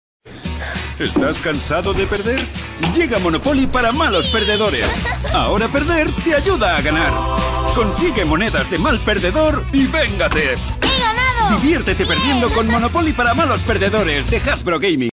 Monopoly Malos Perdedores (Anuncio de Juguetes Hasbro Gaming)
Un Anuncio de Juego de Mesa con Monopoly Malos Perdedores de Hasbro Gaming